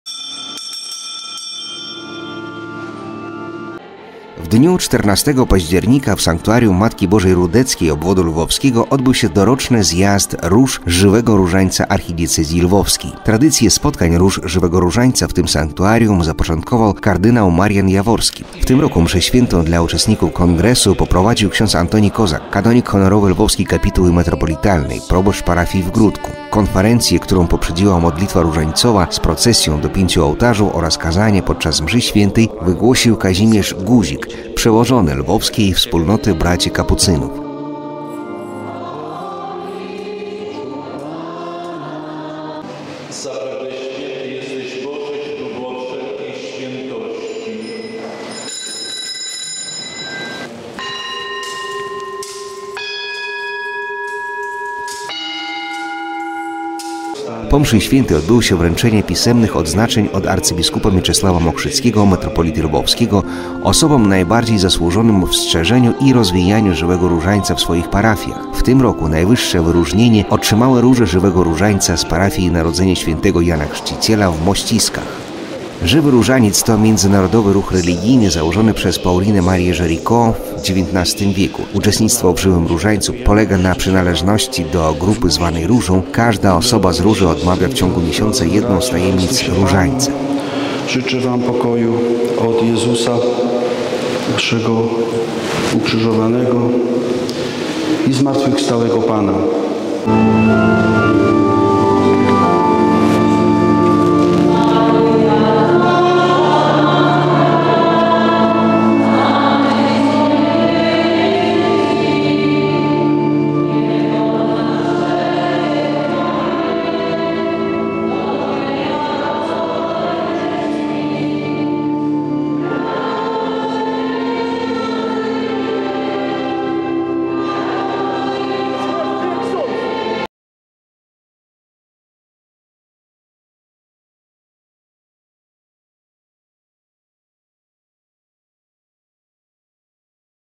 Archidiecezjalny Kongres Róż Różańcowych w Rudkach
W dniu 14 października 2023 roku w Sanktuarium Matki Bożej Rudeckiej obwodu lwowskiego odbył się doroczny Zjazd Róż Żywego Różańca Archidiecezji Lwowskiej.

Tradycję spotkań Róż Żywego Różańca w tym sanktuarium zapoczątkował kardynał Marian Jaworski. W tym roku Mszę Świętą dla uczestników kongresu poprowadził ks. Antoni Kozak Kanonik Honorowy Lwowskiej Kapituły Metropolitalnej, proboszcz parafii w Gródku. Konferencję, którą poprzedziła modlitwa różańcowa z procesją do pięciu ołtarzy oraz kazanie podczas Mszy św., wygłosił br. Kazimyr Guzik OFMCap, przeołony lwowskiej wspólnoty braci kapucynów.

Po Mszy Świętej odbyło się wręczenie pisemnych odznaczeń od arcybiskupa Mieczysława Mokrzyckiego, metropolity lwowskiego, osobom najbardziej zasłużonym w szerzeniu i rozwijaniu Żywego Różańca w swoich parafiach. W tym roku najwyższe wyróżnienie otrzymały Róże Żywego Różańca z parafii Narodzenia św. Jana Chrzciciela w Mościskach.

Żywy Różaniec to międzynarodowy ruch religijny założony przez Paulinę-Marię Jaricot w XIX wieku. Uczestnictwo w Żywym Różańcu polega na przynależności do grupy zwanej różą. Każda osoba z róży odmawia w ciągu miesiąca jedną z tajemnic różańca. Po zmianie, której dokonał św. Jan Paweł II, na jedną różę przypada 20 osób, a nie jak dotychczas 15. W 1832 roku papież Grzegorz XVI w liście apostolskim zatwierdził Wspólnotę Żywego Różańca, udzielił jej licznych dyspens, a jej patronką mianował rzymską męczennicę Filomenę.